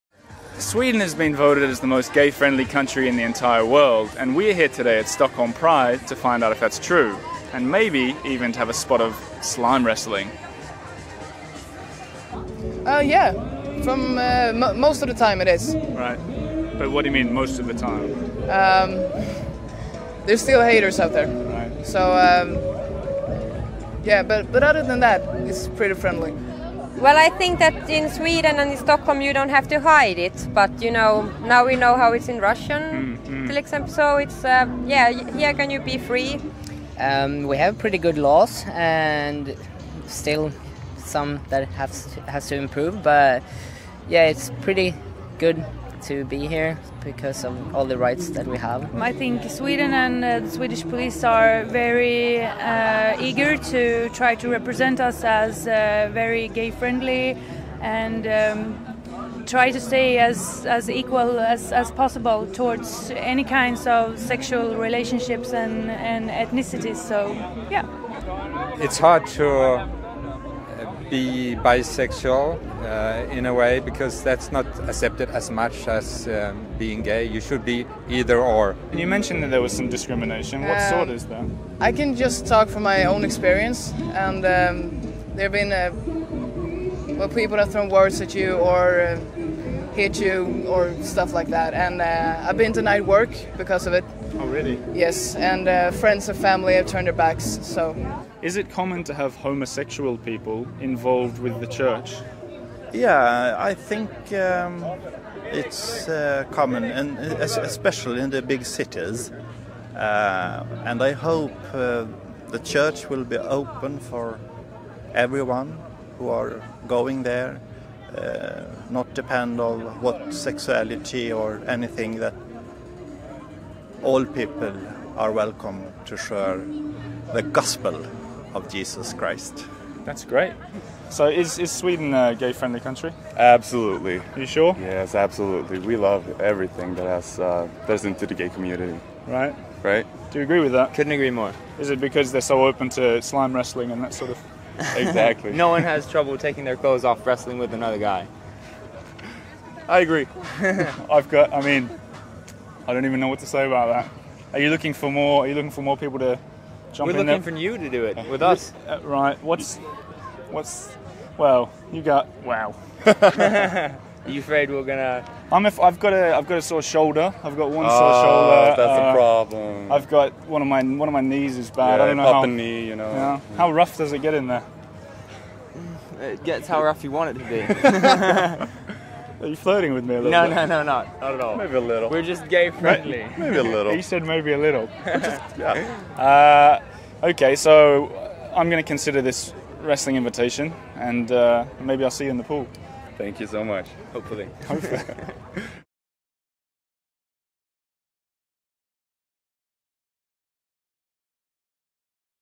Is Sweden the most gay-friendly country in the world?
Sweden is regularly rated as the world's most gay-friendly nation but is its reputation actually true? The Local's Oliver Gee heads to Stockholm Pride to find out. Will he take up an invitation to partake in a spot of gay-friendly slime wresting? Watch and see!